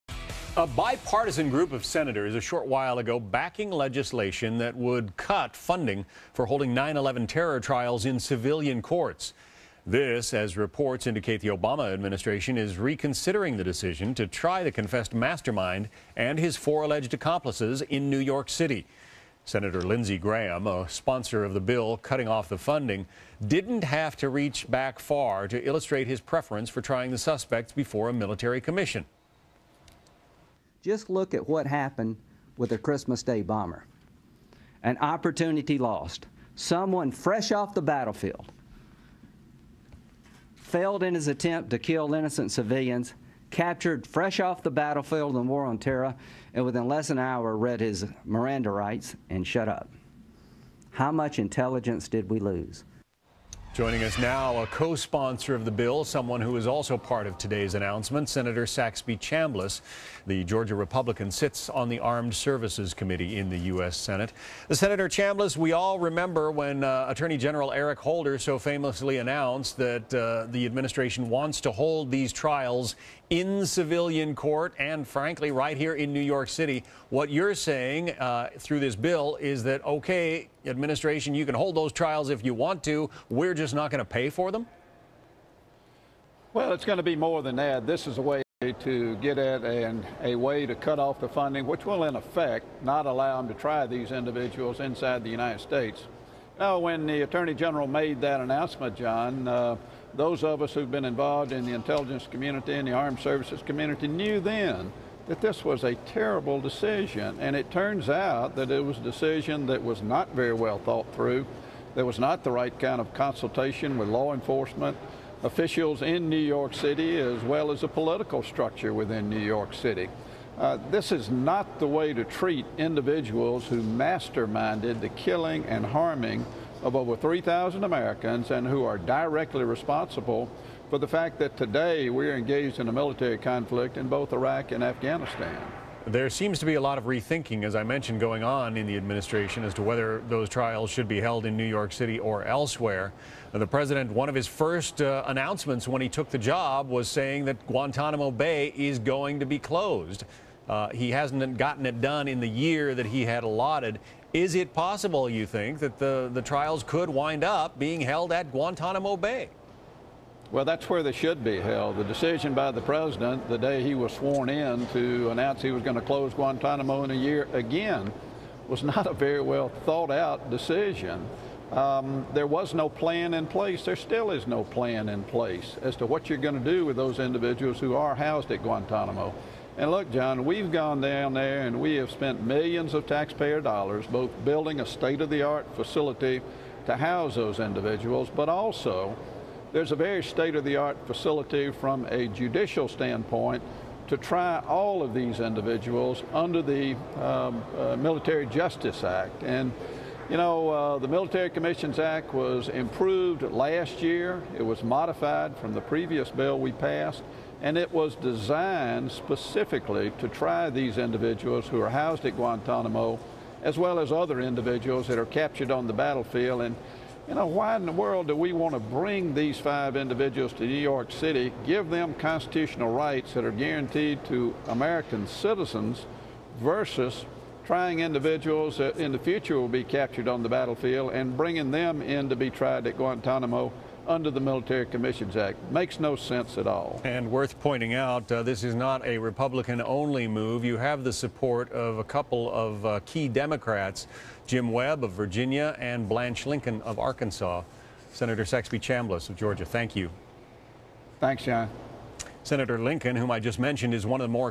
Sen. Chambliss Introduces Bi-Partisan Bill To Block Gitmo Transfers
Sen. Saxby Chambliss Introduces Bi-Partisan Bill To Block Gitmo Transfers